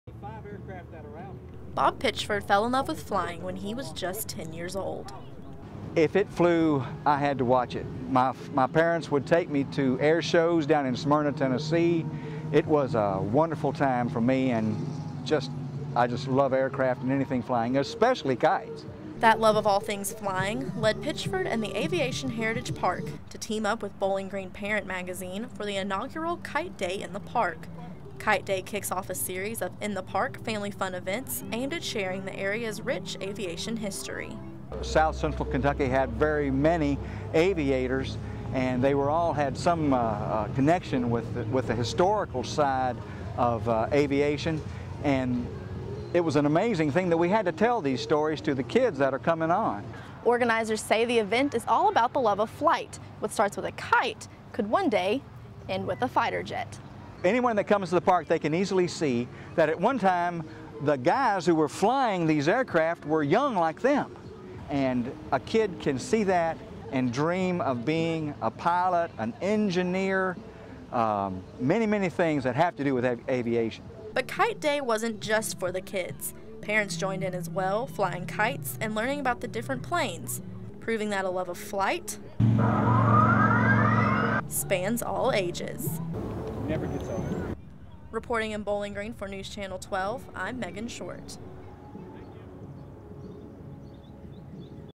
Kite Day at the Park
The Aviation Heritage Park and Bowling Green Parent Magazine paired up to inspire the next generation of aviators.